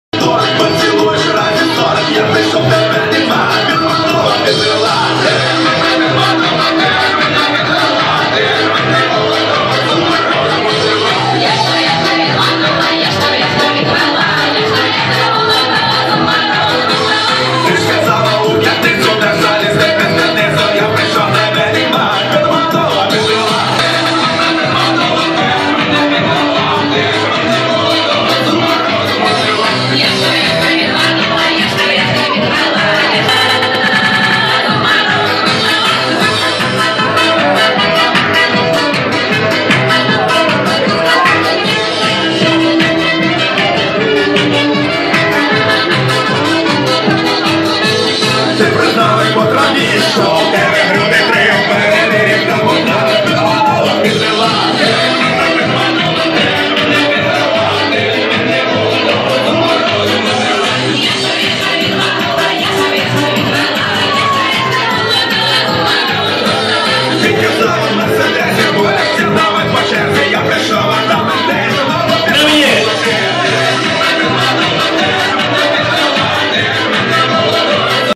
Uò Carnv'l - Arci Cafiero 2013
Festa di Carnevale in maschera all'Arci Carlo Cafiero di Barletta